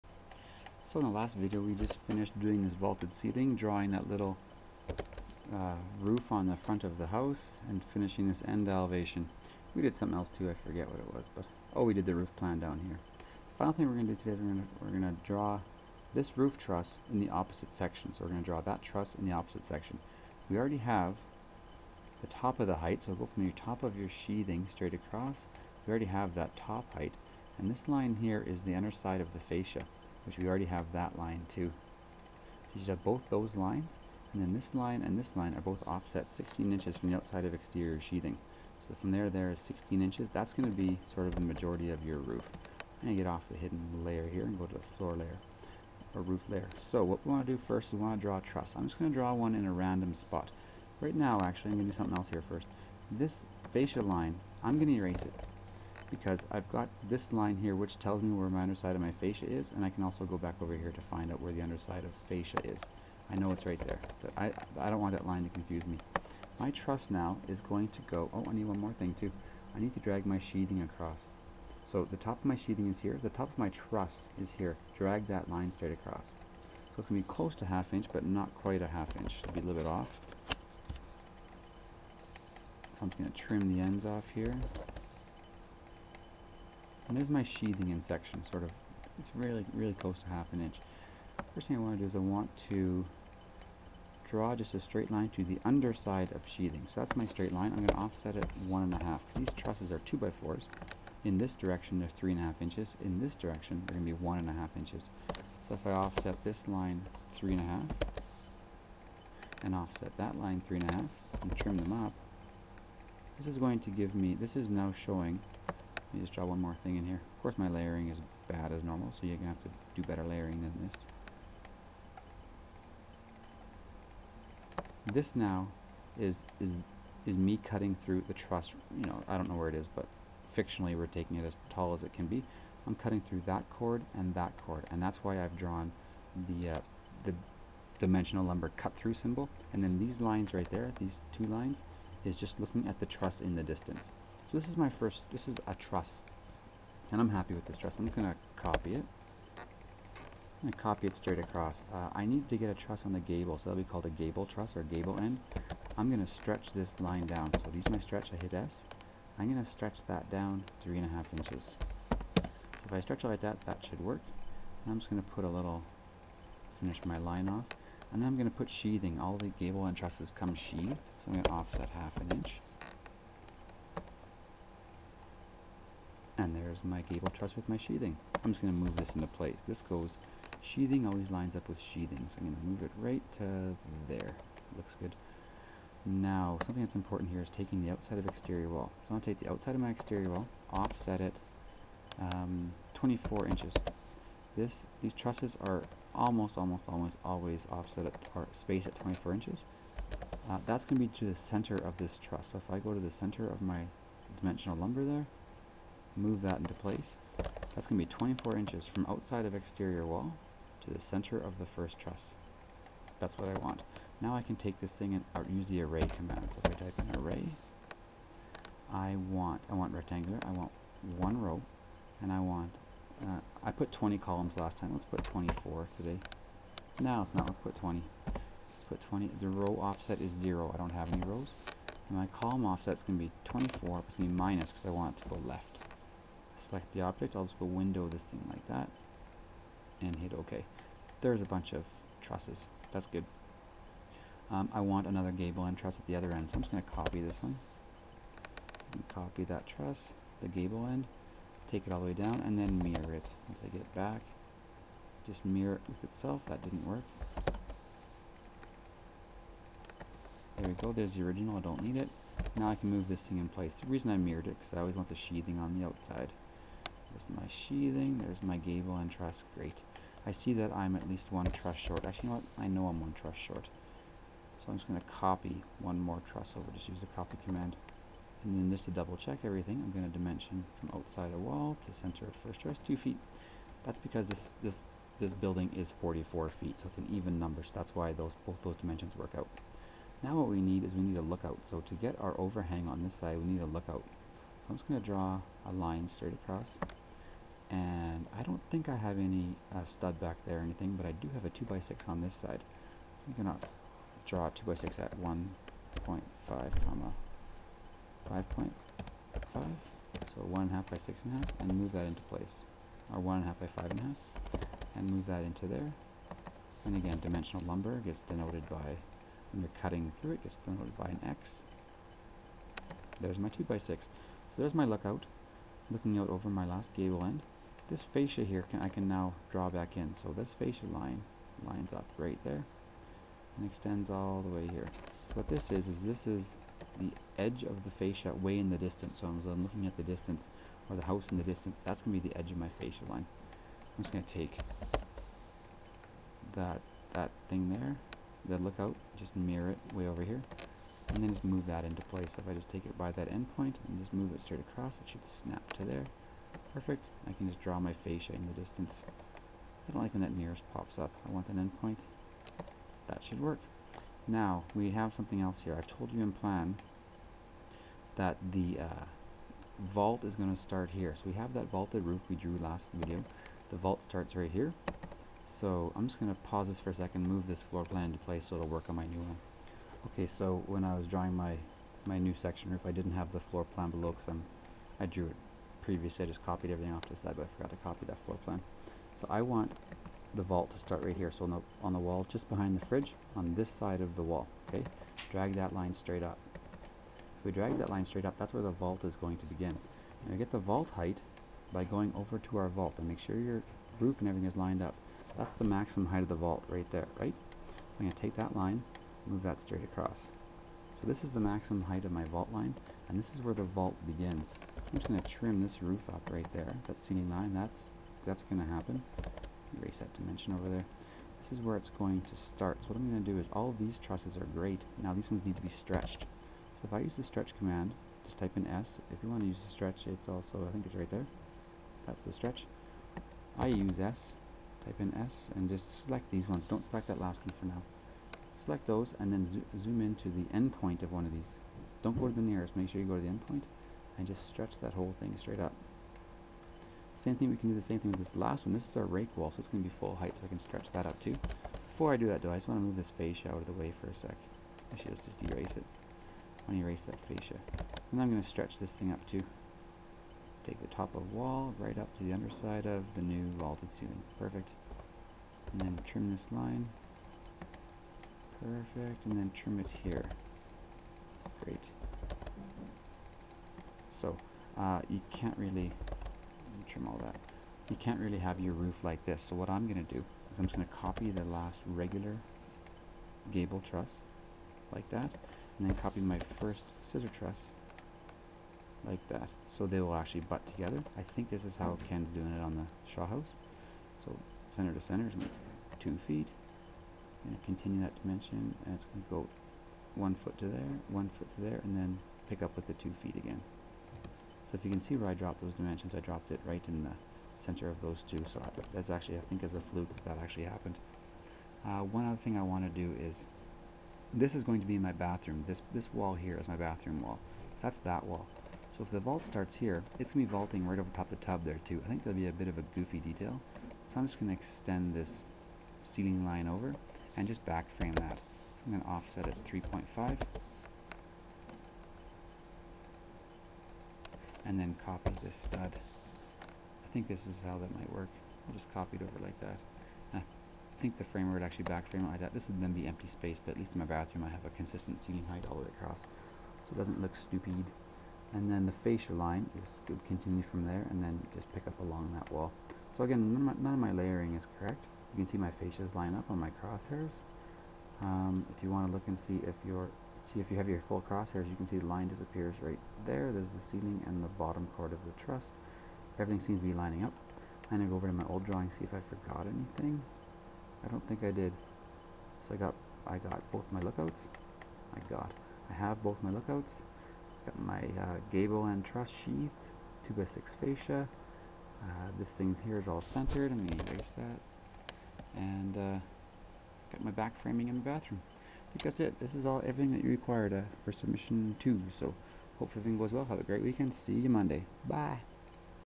How to draw a house in AutoCAD, Video 10 - Long section
This video shows you how to draw the roof trusses in the long section.